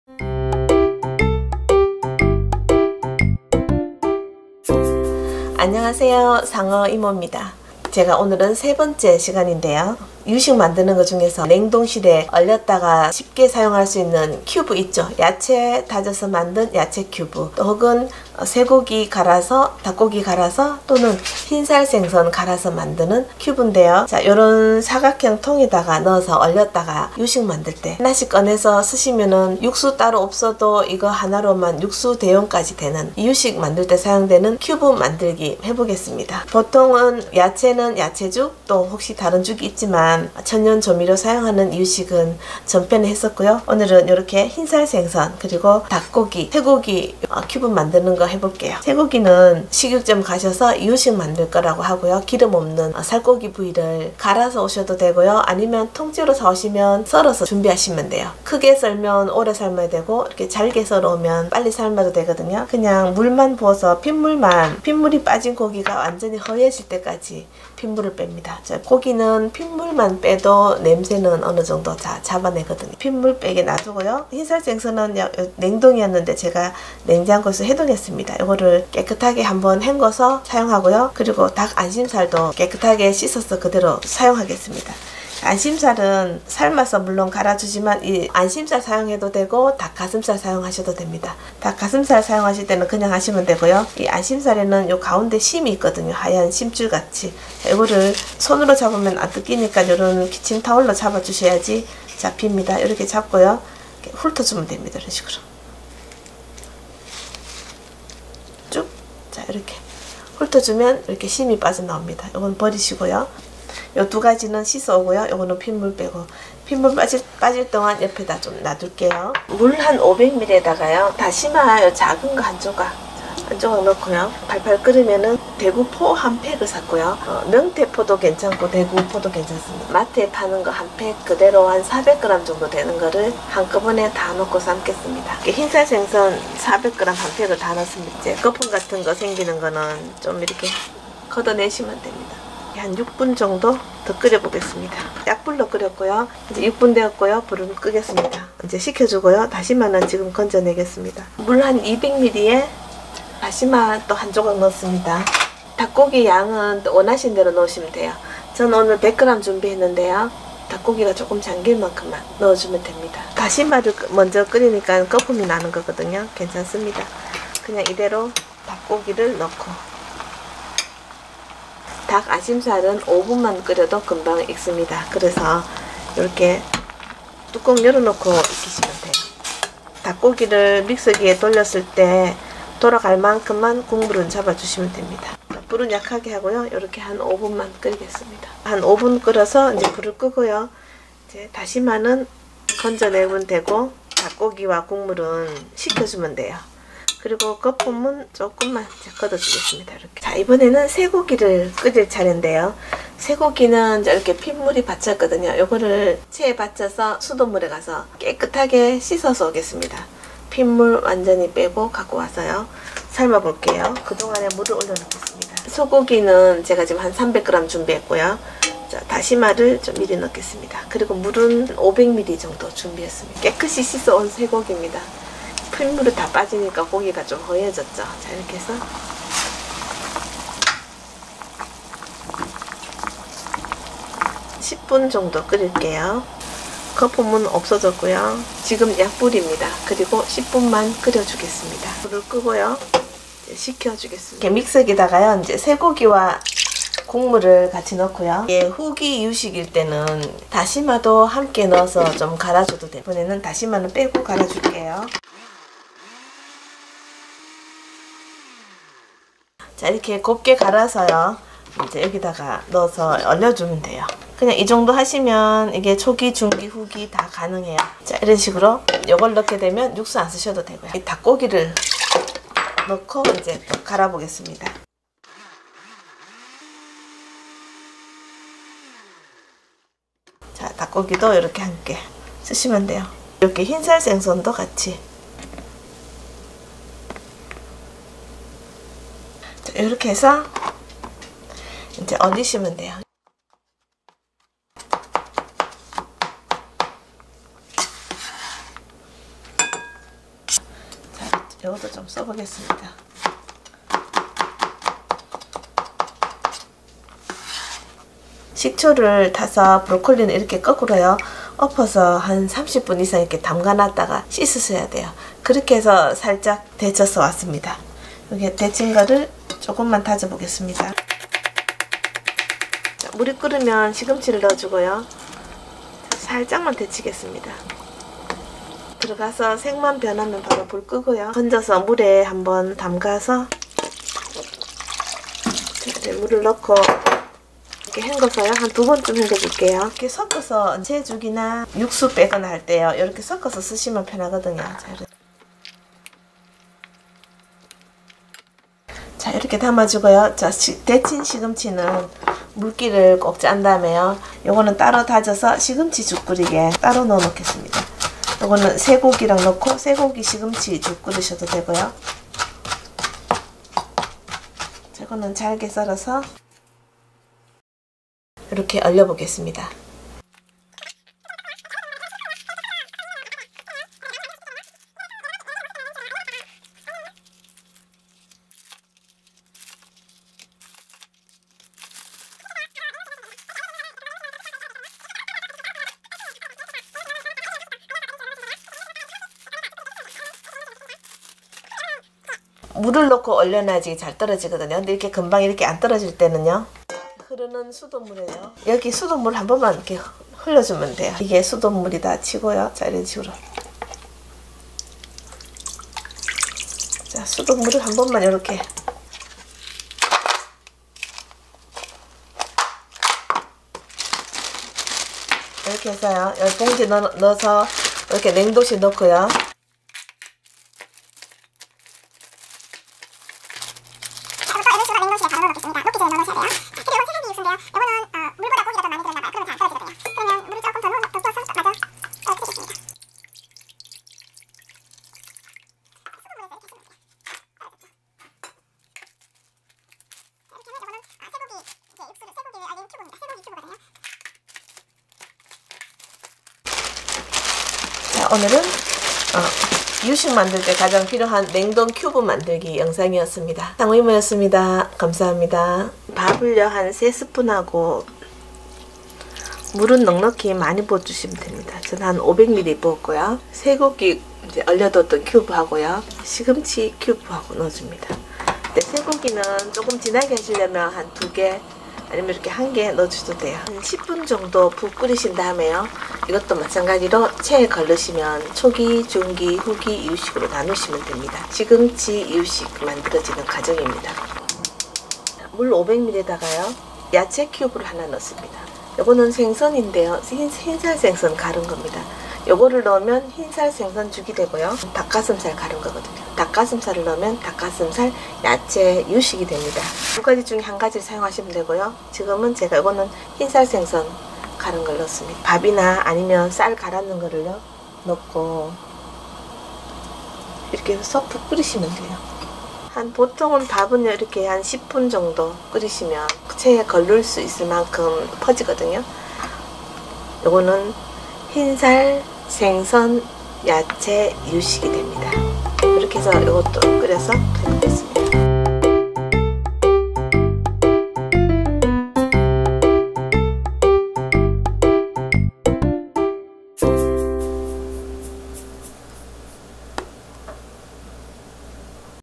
이유식만들기중 냉동큐브입니다 [상어이모.SANGEOIMO]
안녕하세요?
오늘은 이유식만들기 3번째 시간
냉동큐브만들기입니다
쇠고기
흰살생선
닭슴살이나 닭안심살
그리고야채큐브입니다